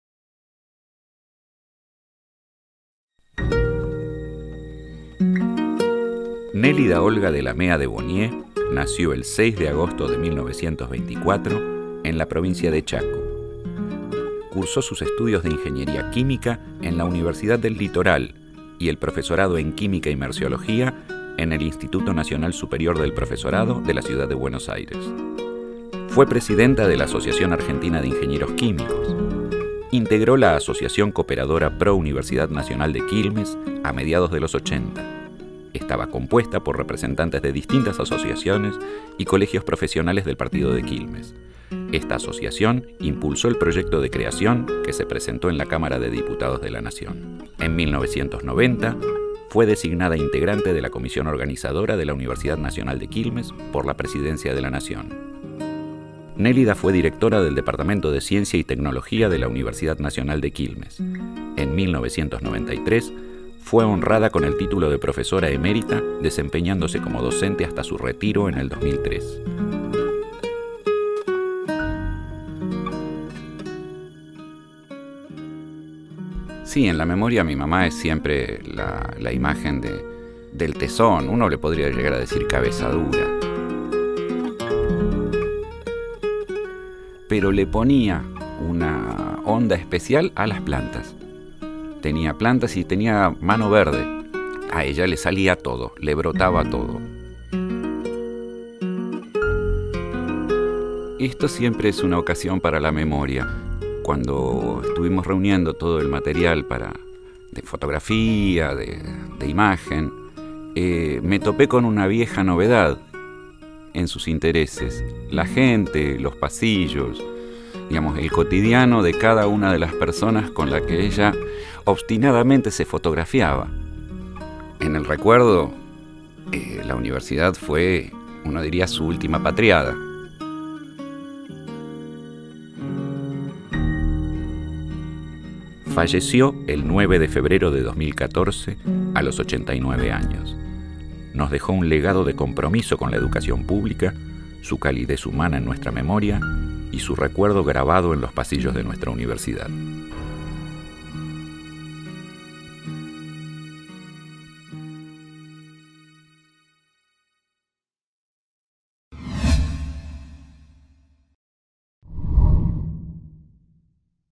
Homenaje a la Ingeniera Nélida Dellamea de Bonnier
El jueves 12 de diciembre de 2019 a las 16 se realizará el acto de descubrimiento de una placa en homenaje a la Ingeniera Nélida Bonnier en las aulas del Edificio Espora de nuestra universidad.

Nélida fue una de las autoras del proyecto de creación de la Universidad Nacional de Quilmes y, en consecuencia, fue designada integrante de su Comisión Organizadora. Su labor como docente de Matemáticas, Física y Química alcanzó a la mayoría de los colegios de Quilmes, incluyendo al Nacional, la Inmaculada Concepción, el Comercial, el Nazareth, y el Normal Superior, entre otros.

En la Universidad, se desempeñó como docente del Departamento de Ciencia y Tecnología hasta su retiro en 2003, cuando fue distinguida con el título de Profesora Emérita.